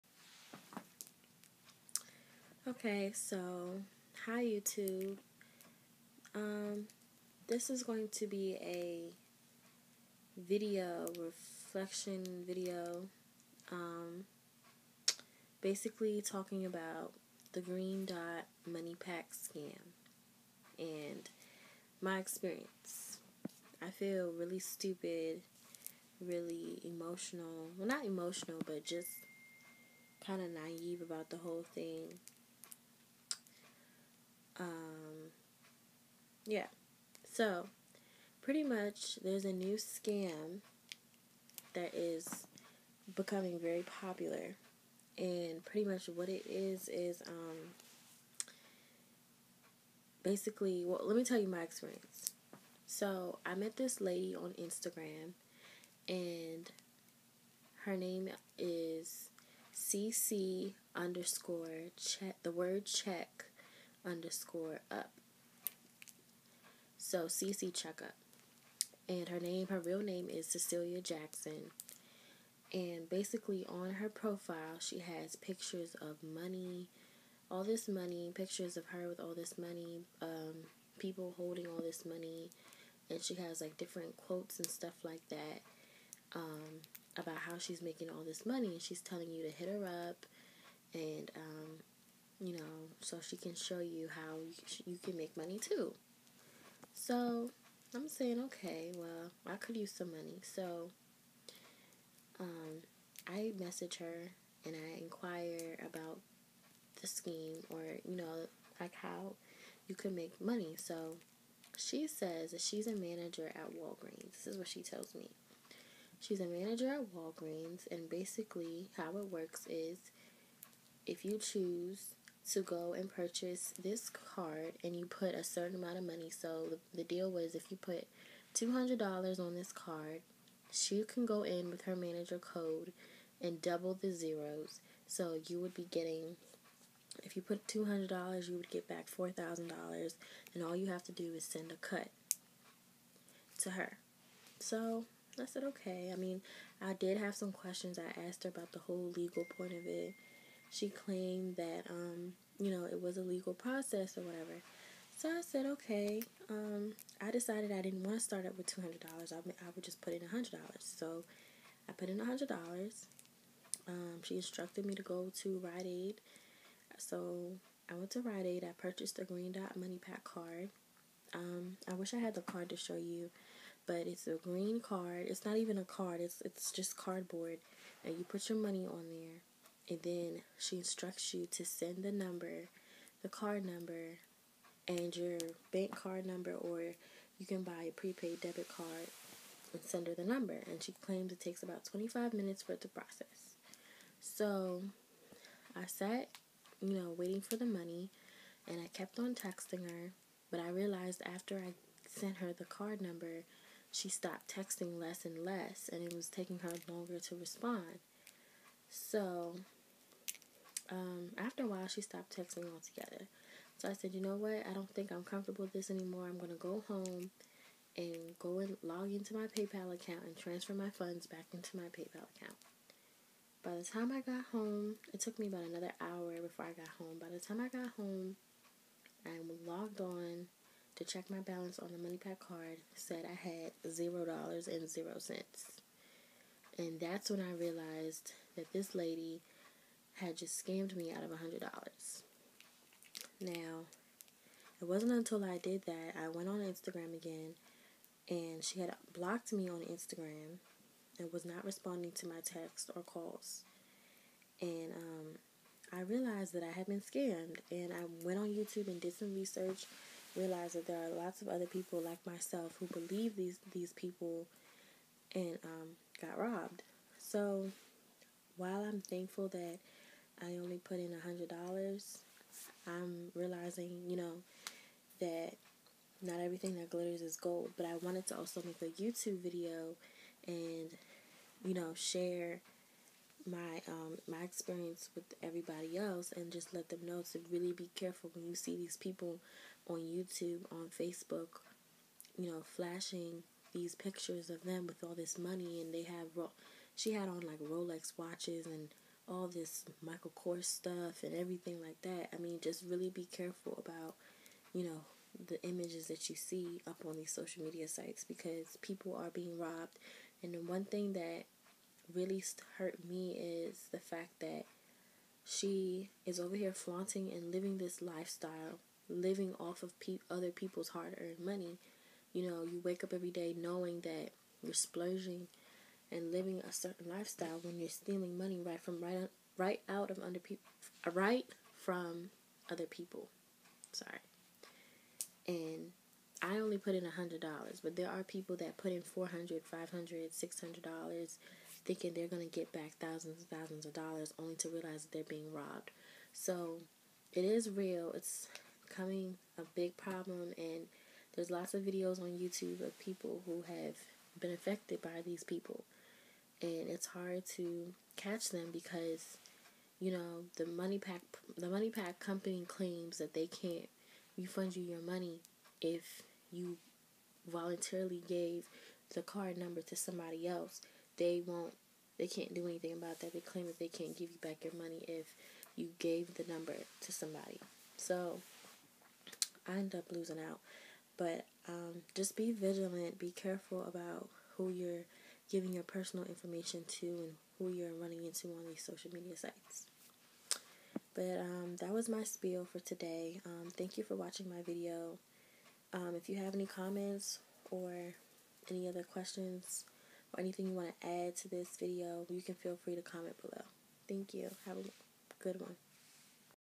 BEWARE!: GREENDOT MONEYPAK CARD SCAM!!!!!
BEWARE! New scam that's becoming prominent all over the country. Stay focused guys!